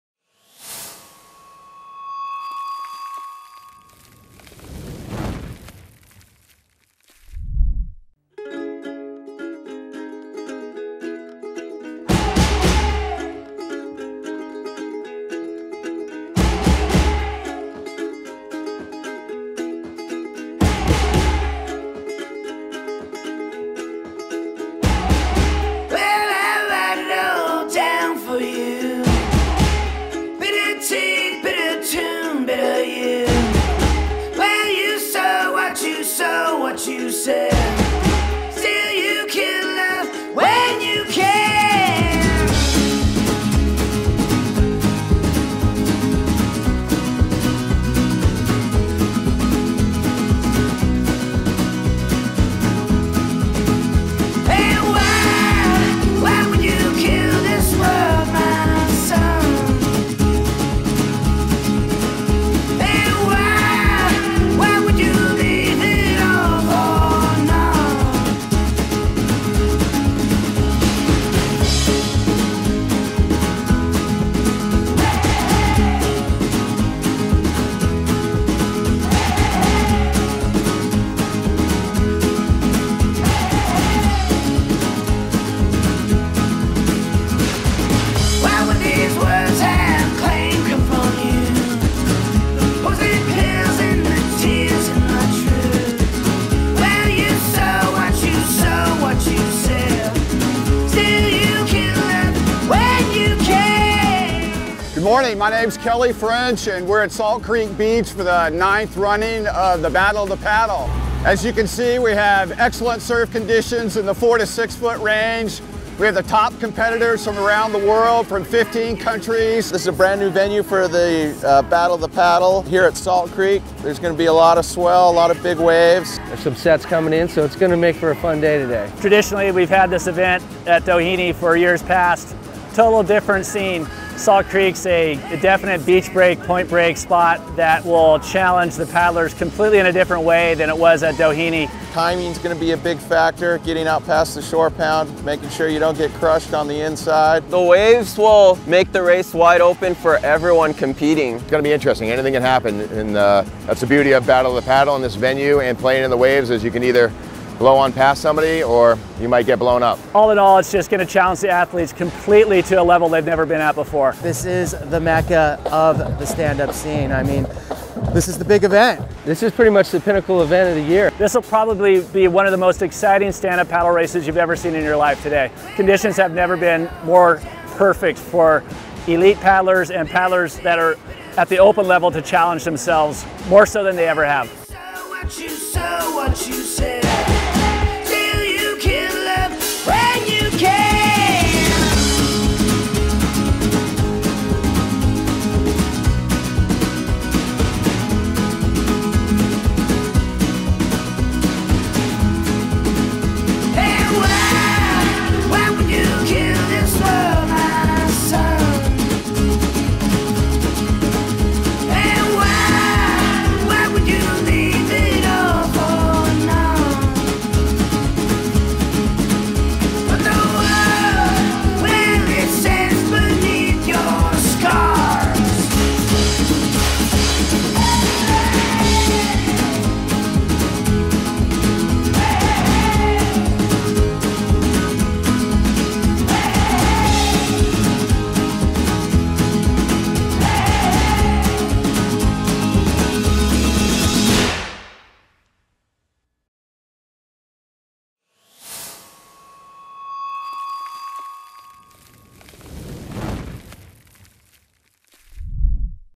Battle of the Paddle 2014 Final at Salt Creek Beach in Dana Point, CA
As we always do, SIC had the film crew on location for the Battle of the Paddle 2014 final at Salt Creek Beach in Dana Point, CA . Here is a quick look at went down. for more information on the 2014 SIC stand up paddleboards.